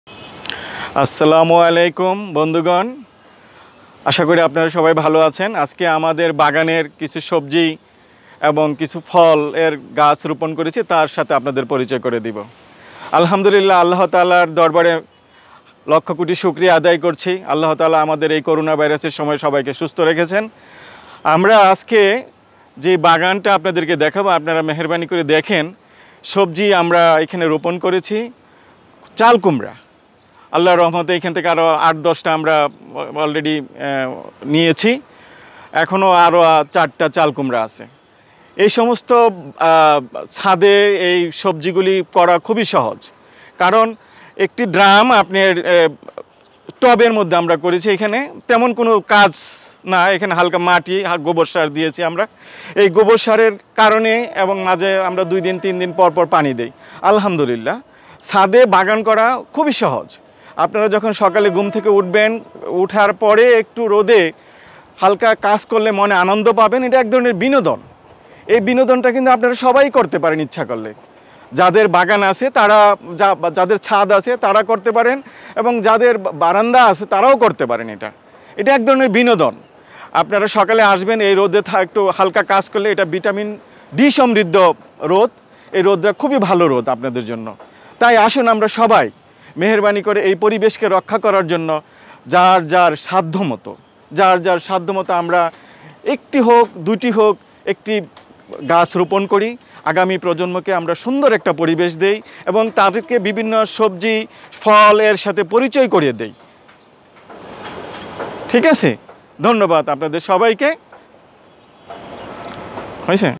ছাদে চাল কুমড়ার সুন্দর বাগান। ছাদে বাগান করুন আনন্দের সাথে, দেহ- মন সুস্থ্য রাখুন।
ছাদে বাগান করুন আনন্দের সাথে, দেহ- মন সুস্থ্য রাখুন। চাল কুমড়ার সুন্দর বাগান।